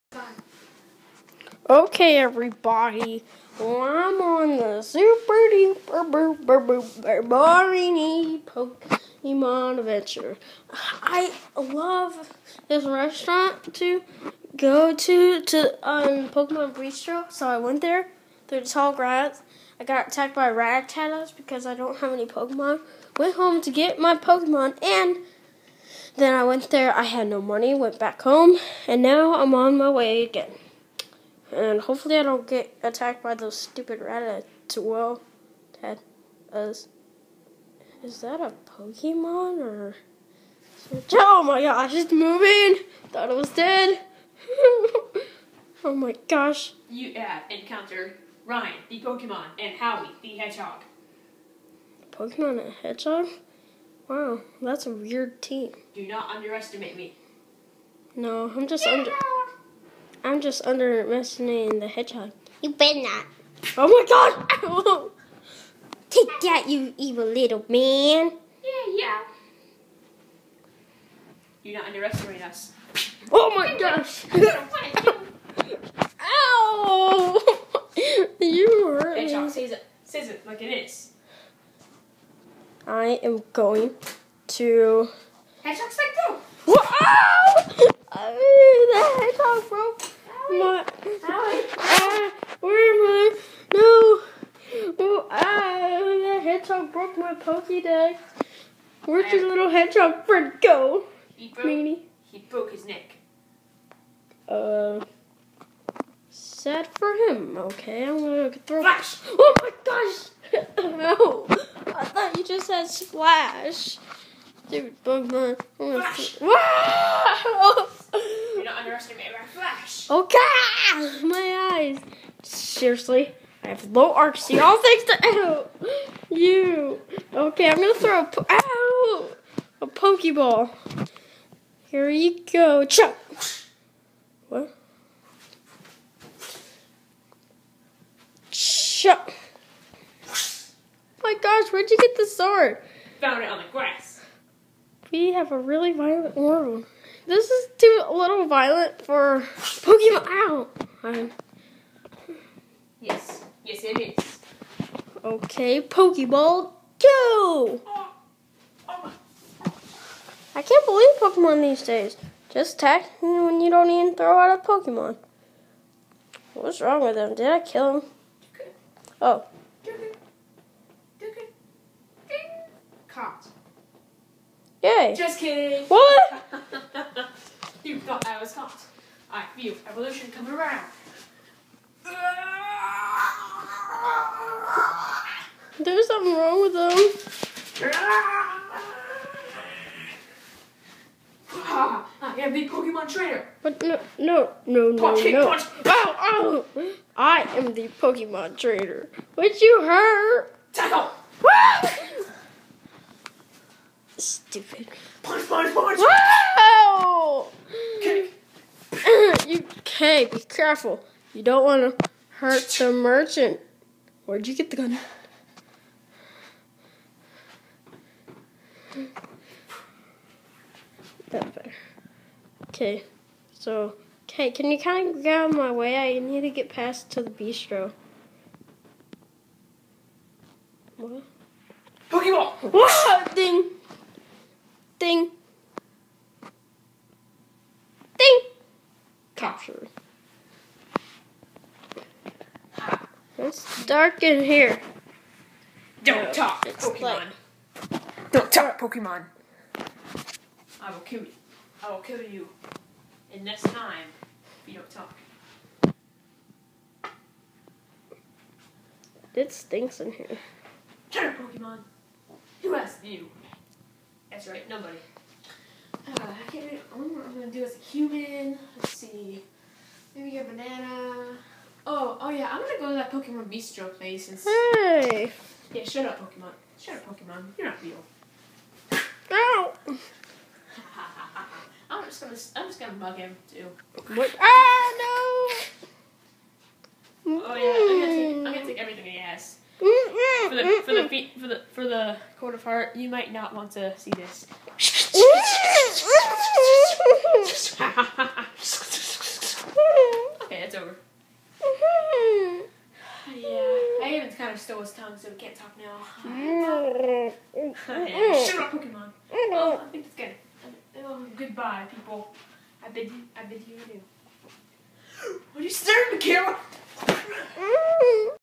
Pokemon battle sequence in real life
A random person comes along and sees a Pokemon with a hedgehog. A battle ensues. By the way,the Pokemon was just killing him, by the way.